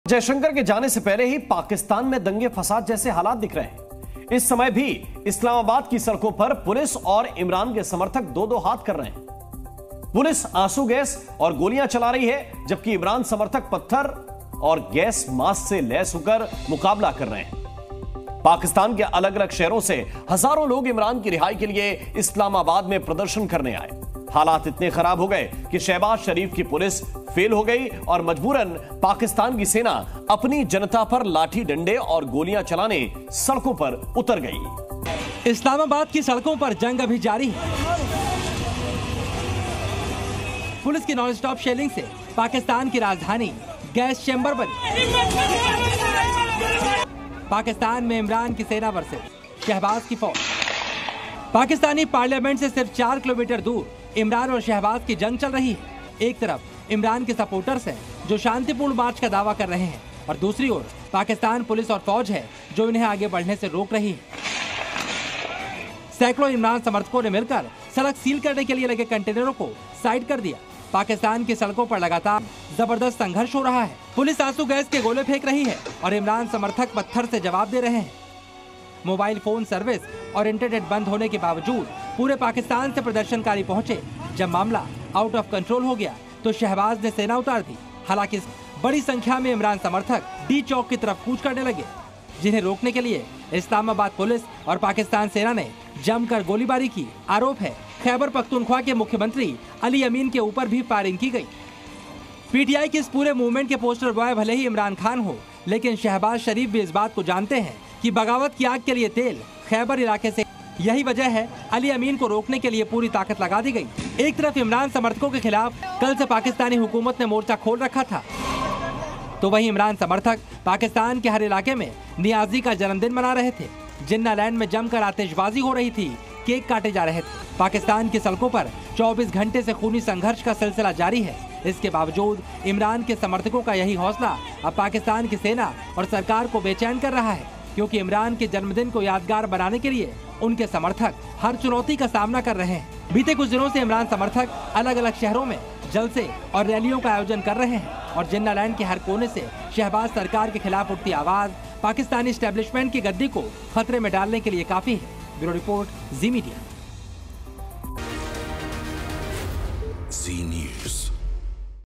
Pakistan War Update: 'युद्ध' शुरू..पाकिस्तान में उतरी फौज! | Riots |Imran Khan Crisis | S Jaishankar
जयशंकर के जाने से पहले ही पाकिस्तान में दंगे-फसाद जैसे हालात दिख रहे हैं...इस समय भी इस्लामाबाद की सड़कों पर पुलिस और इमरान के समर्थक दो-दो हाथ कर रहे हैं..पुलिस आंसू गैस और गोलियां चला रही है..जबकि इमरान समर्थक पत्थर और गैस मास्क से लैस होकर मुकाबला कर रहे हैं..पाकिस्तान के अलग-अलग शहरों से हजारों लोग इमरान की रिहाई के लिये इस्लामाबाद में प्रदर्शन करने आए..हालात इतने खराब हो गये कि शहबाज शरीफ की पुलिस फेल हो गई..और मजबूरन पाकिस्तान की सेना अपनी जनता पर लाठी डंडे और गोली चलाने सड़कों पर उतर गई

Even before Jaishankar's departure, riot-like conditions are visible in Pakistan... even now, police and Imran's supporters are fighting on the streets of Islamabad... police is firing tear gas and bullets... while Imran's supporters are fighting back armed with stones and gas masks... thousands of people from different cities of Pakistan came to protest in Islamabad for Imran's release... the situation became so bad that Shahbaz Sharif's police failed... and Pakistan's army was forced to come on the streets to use sticks and bullets on its people